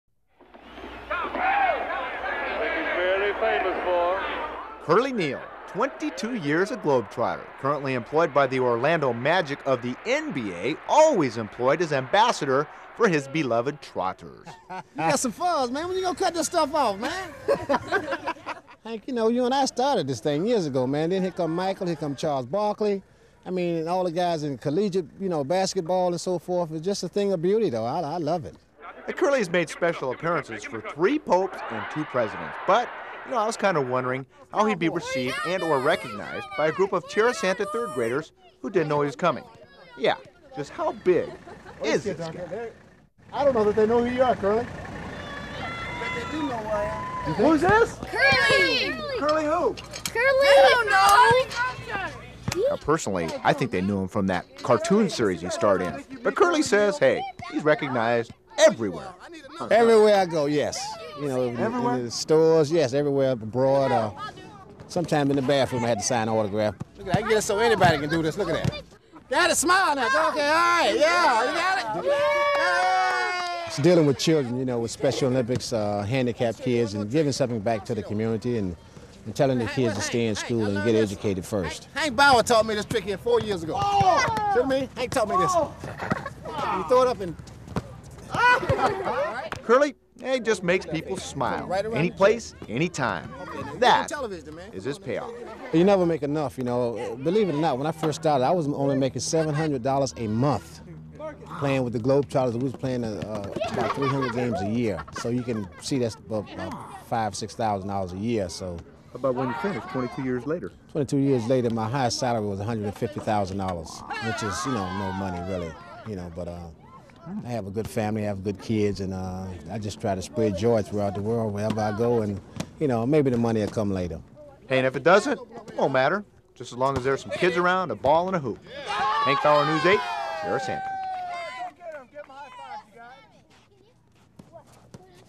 Curly Neal Harlem Globetrotter 1993
Curly Neal died March 26, 2020. He was 77 years old.

News 8's Hank Bauer was with him in January 1993 when he thrilled Tierrasanta third graders with his signature moves. What a treat it must have been for those boys. I'm sure they remember that day. The Globetrotters were in town for a show at the Sports Arena.